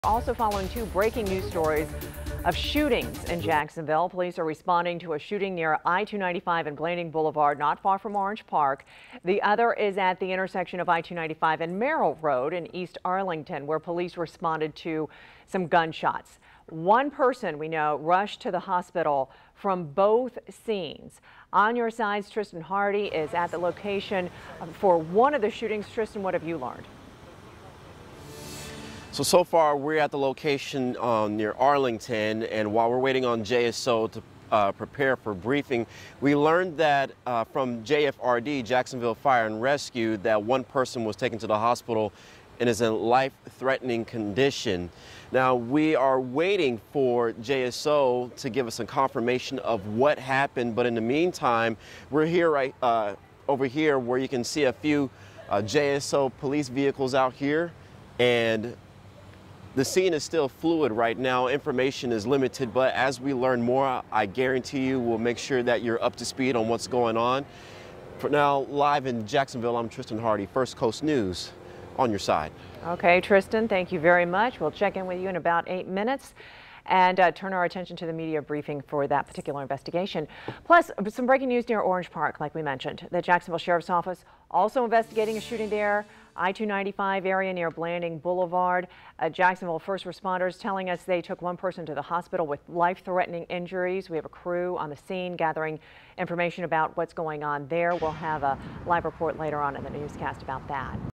JSO responding to two shootings in Jacksonville
One scene is in the 6000 block of Youngerman Circle while the other is at I-295 and Merrill Road.